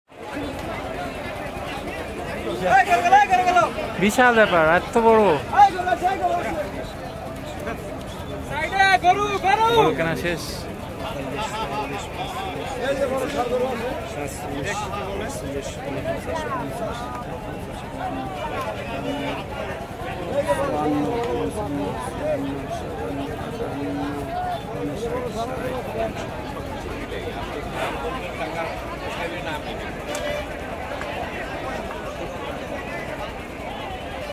Gabtoli Gorur haat- 80k big giant cow purchased by Robin
Gabtoli hut- 80k big giaqnt cow purchased by Robin